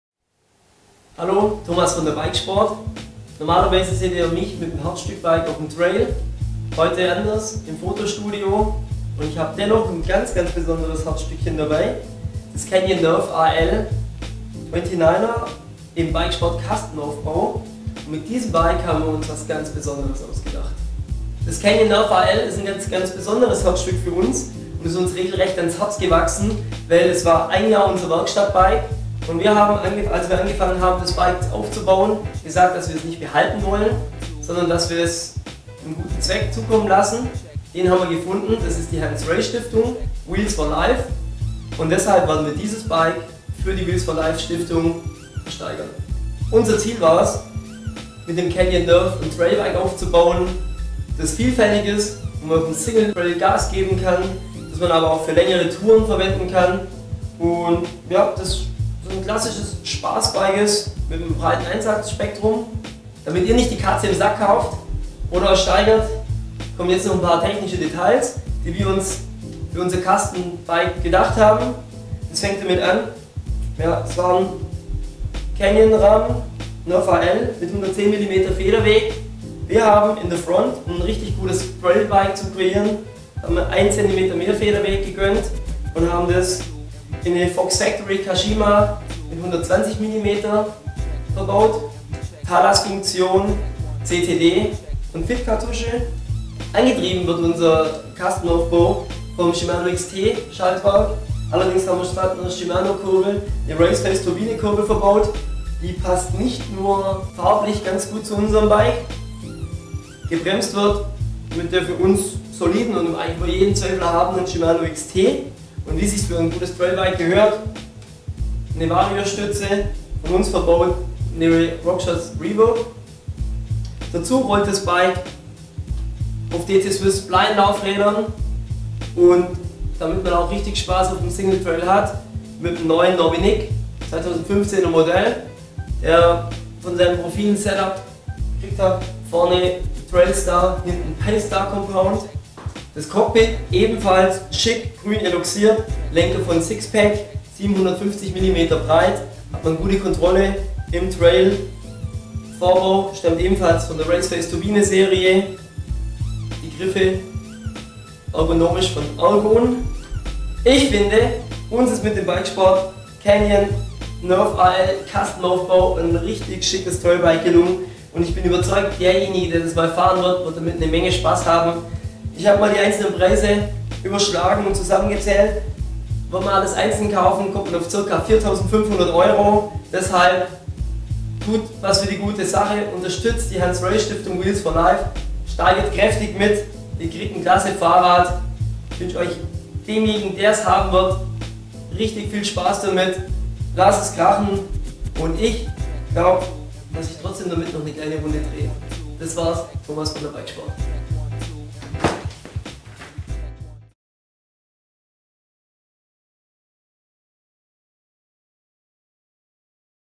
bikesport Magazin Herzstück: Test - Canyon Nerve AL 29 Custom Version
Wir stellen vor im Test: Das Canyon Nerve AL 29er in unserer Custom Version. Dieses wird von uns ab dem 10. Dezember auf ebay versteigert.

Redaktion: Thomas Werz
Video: Gideon Heede